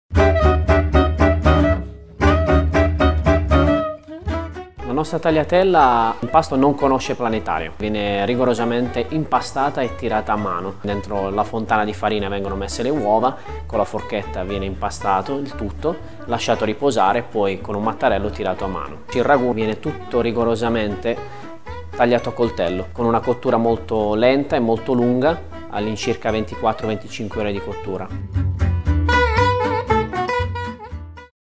Tagliatelle al ragù - L'Erba del Re - Racconta la tua Città
Ci sono tanti modi di cucinare i piatti tipici della cucina modenese, ognuno ha la sua variante. Questa è quella di Pietro Gisoldi del ristorante L'Erba del Re. Segui tutte le ricette, le varianti ed i video degli chef su